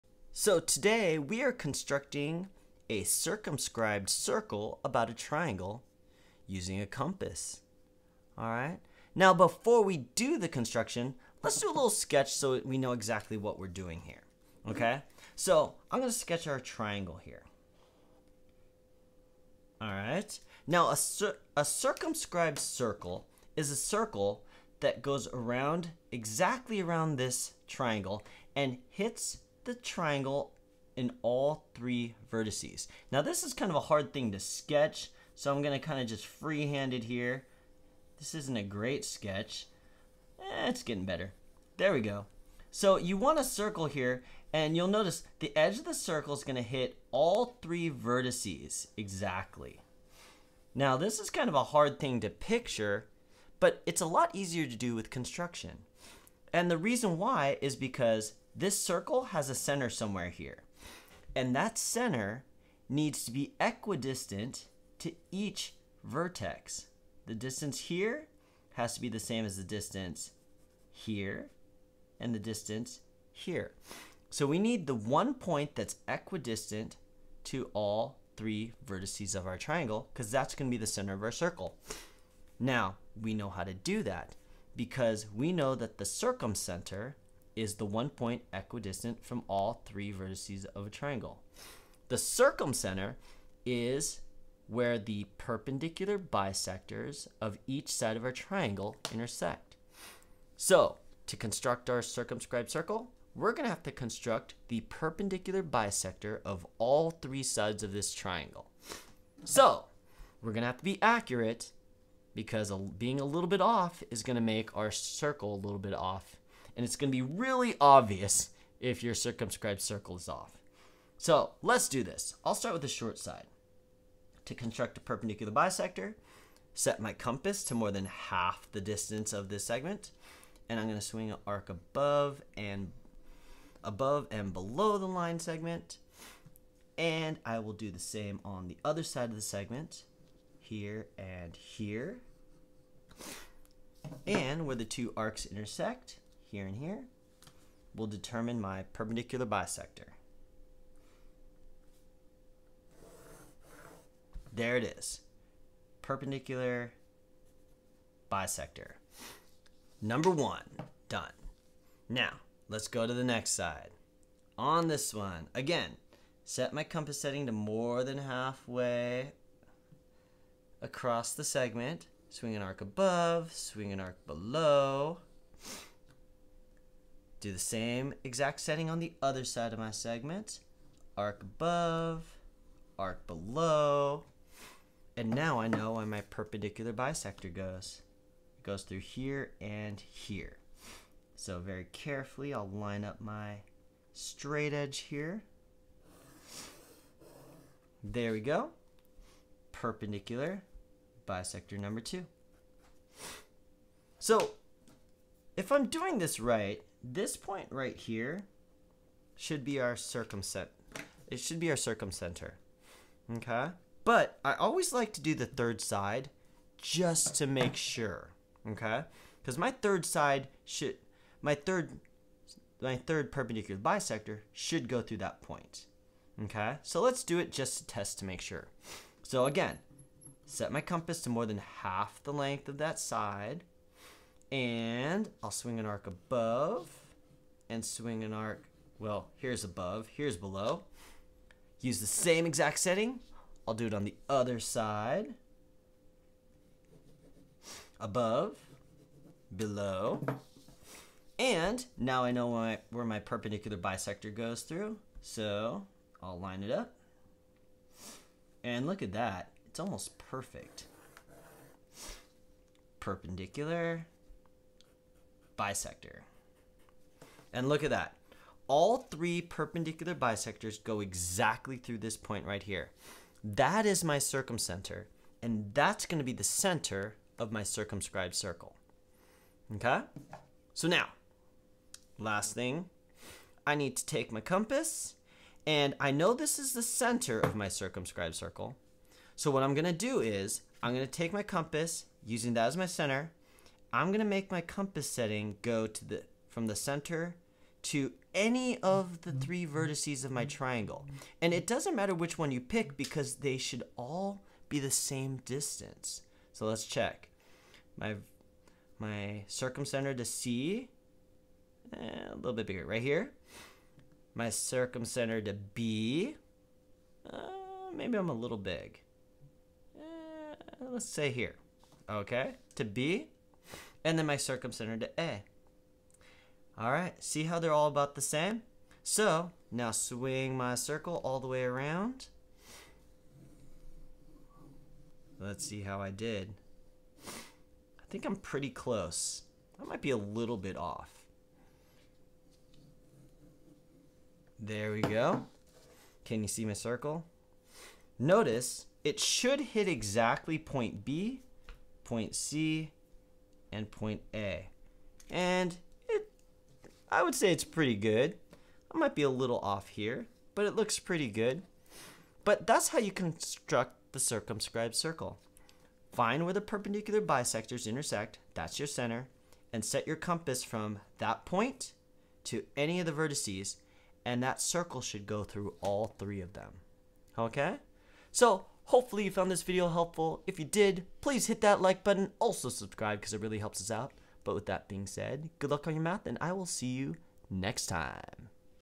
Constructing a Circumscribed Circle
Constructions can be confusing!  In this video, I show you how to construct a circumscribed circle around a triangle.